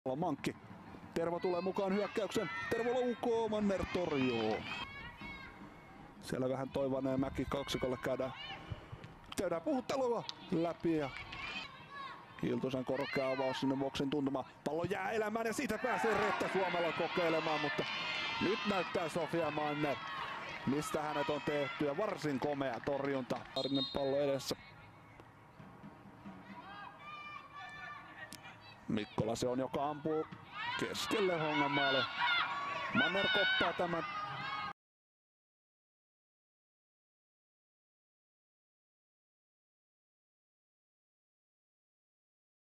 Sofia Manner, kolme ottelua, kaksi nollapeliä!
Kolme ottelua, kaksi nollapeliä!

Ei hassummat tilastot! Sofia Mannerin nolla pysyi eilen PK-35 Vantaata vastaan mm. näillä torjunnoilla!